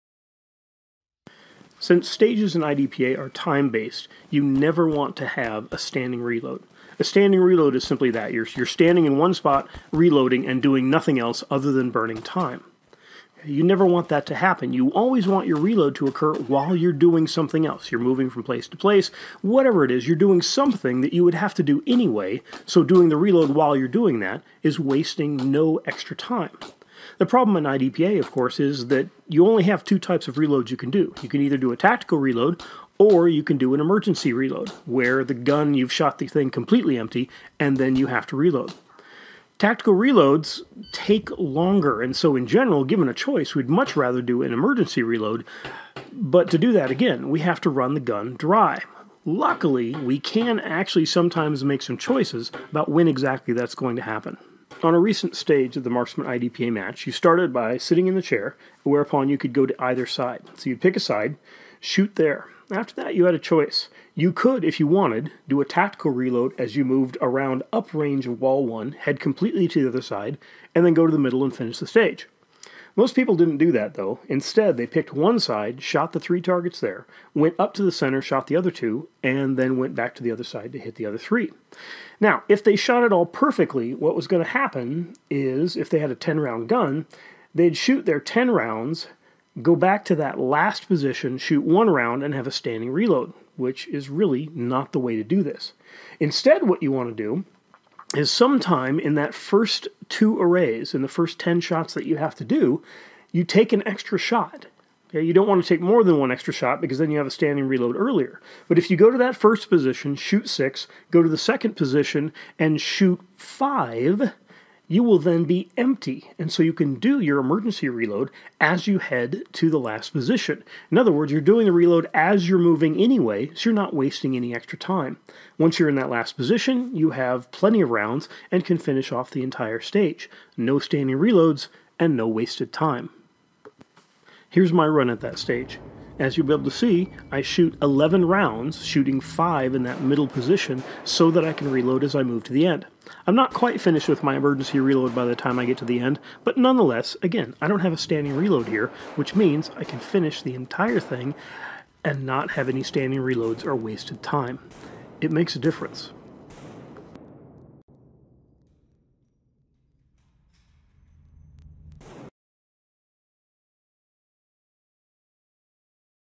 IDPA Fundamentals: Setting up an Emergency Reload
Since you can only perform two types of reloads in IDPA (and a tac reload is slower than an emergency reload) it is important to know that sometimes, you can set up exactly when you need to perform an emergency reload to be prepared for the next shooting position, and more importantly, NOT have a standing reload.